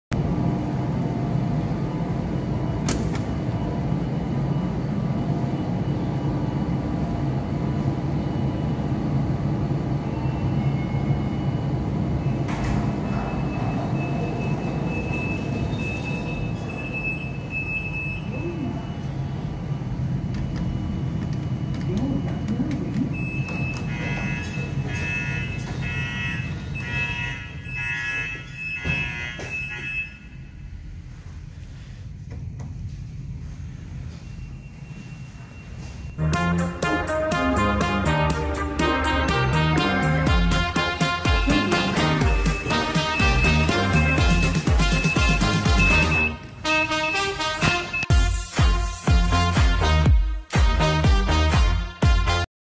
EPIC FAIL! Fujitec goods lifts at Pixel Red
This lift button requires me to use a lot of pressure when pressing. I couldn't press 1 REAR to 5. As the lift went into nudge mode, I kept struggling. Then when the lift door close. The last thing I did was to press 1 FRONT. Then I as reached 1 FRONT floor, the door opened and I struggled pressing B1. As there was someone outside of the lift, I wanted to find where's the passenger lifts. He said that there's a door where I needed to walk left, this way, I can see the passenger lifts. In the end, I thanked him. By the way, the lift doors behave like HDB lift doors with the beeping sound when opening or closing.

Manufacturer: Fujitec
Floors: B1, 1 FRONT, 1 REAR, 2, 3, 4, 5
Capacity: 3000kg
Year installed: 2015
Rating: 35/50
History: Original

Lift filming is not a crime.
Date filmed: 2 September 2022